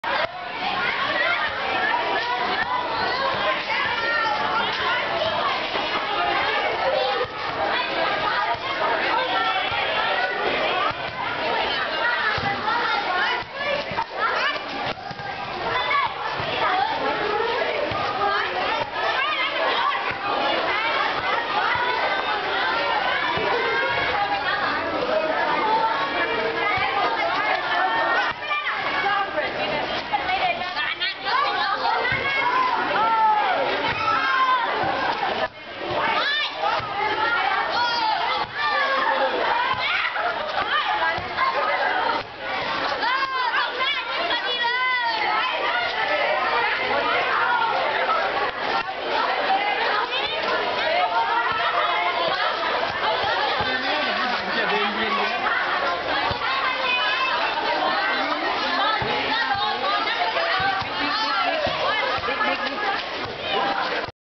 Volunteer in Thailand - Francesca at school
First day volunteering in Thailand with Francesca from California.
Playing volleyball with the students at Takuapa Senanukul school.
Francesca teaches at two primary schools Monday to Thursday, and on Fridays she goes to SENA which is a secondary school.